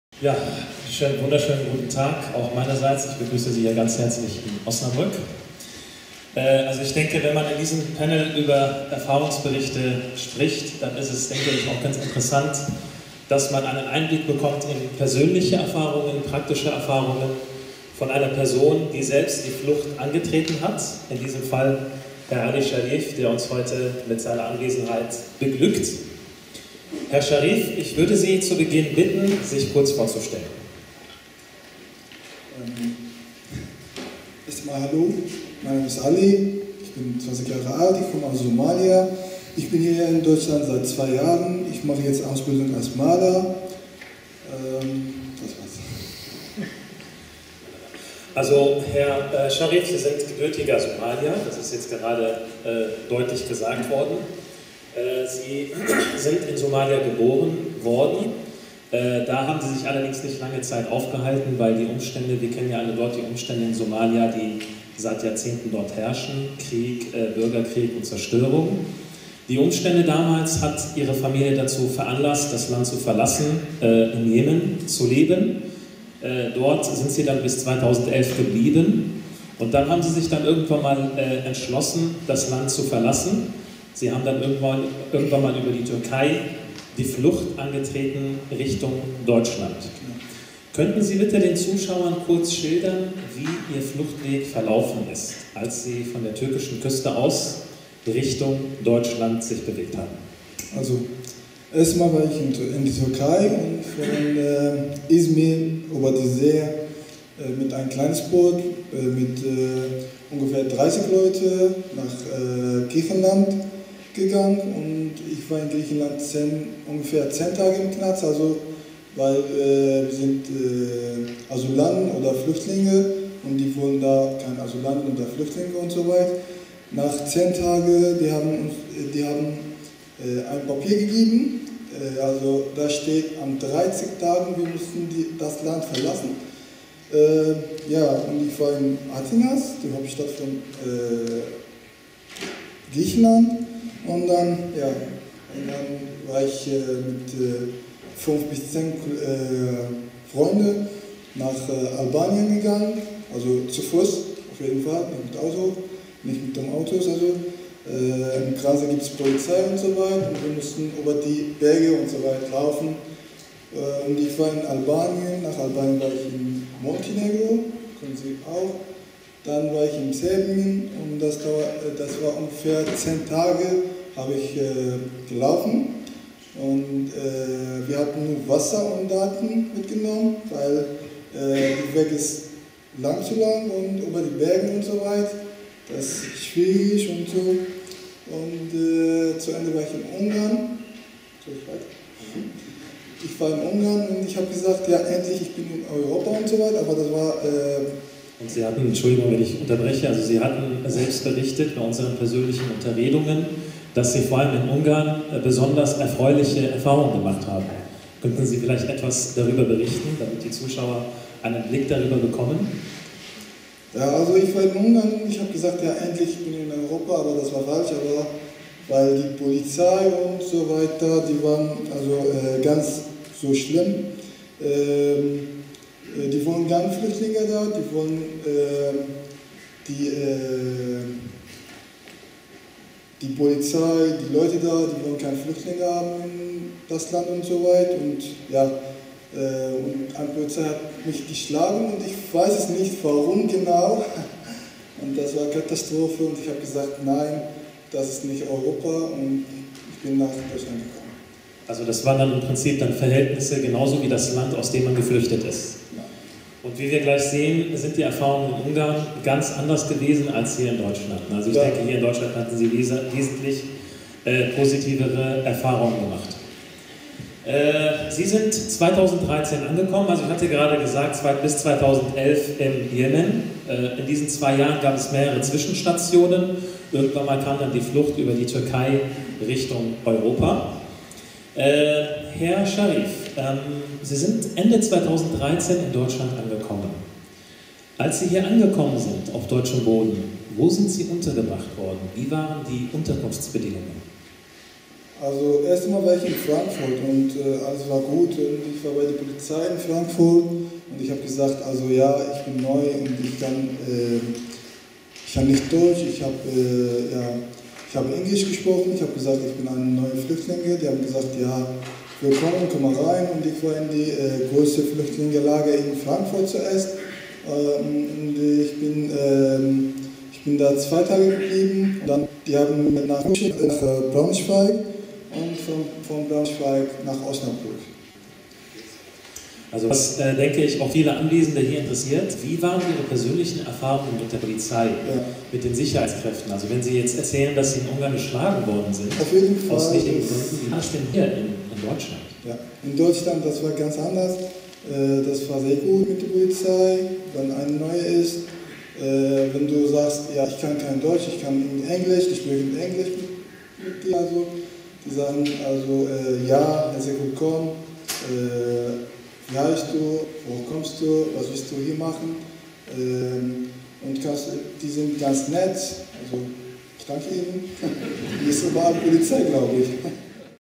Ali Mohamed Sharif spricht über seine Erlebnisse als Flüchtling
Institut für Islamische Theologie, Universität Osnabrück